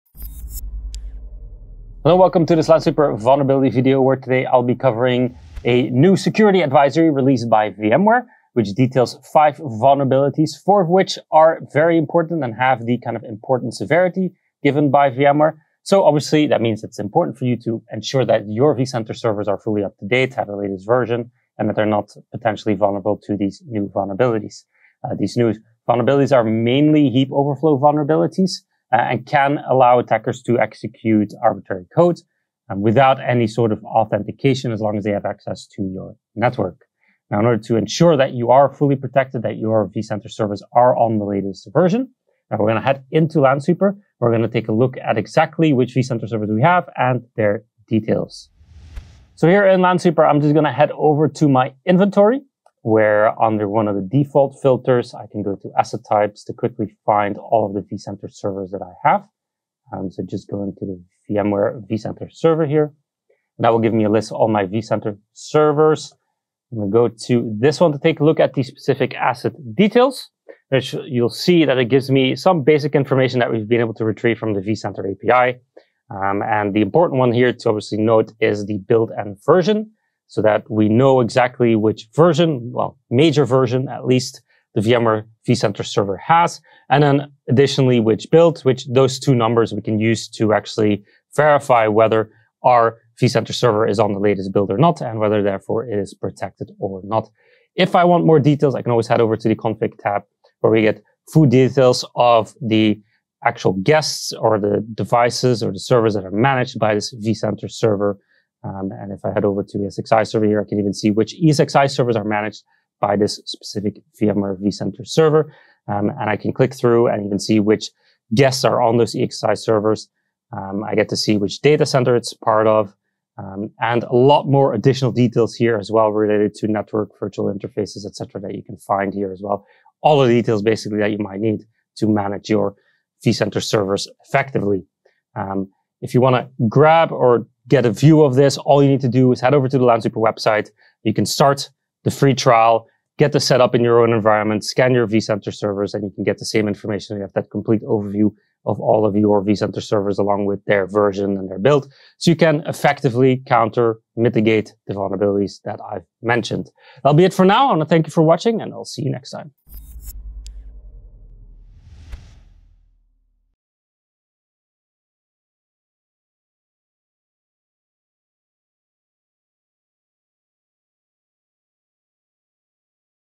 VMware vCenter Server Vulnerabilities CVE-2023
VMware released security updates for vCenter Server in response to a number of memory corruption vulnerabilities. When exploited, the bugs could allow an attacker to execute code or bypass authentication. Use Lansweeper to find any vulnerable installs in your network

VMware's security advisory addresses 5 vulnerabilities in the implementation of the DCERPC protocol, 4 of which are in the important severity range with a CVSSv3 base score of 8.1. CVE-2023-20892 (a heap overflow vulnerability) and CVE-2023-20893 (a use-after-free vulnerability) could allow an attacker to execute arbitrary code on the underlying operating system that hosts vCenter Server. CVE-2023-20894 and CVE-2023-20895, respectively an out-of-bounds write and read vulnerability, could allow an attacker to bypass authentication through memory corruption. A fifth vulnerability, CVE-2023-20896, received a moderate severity rating of 5.9 and could lead to denial of service.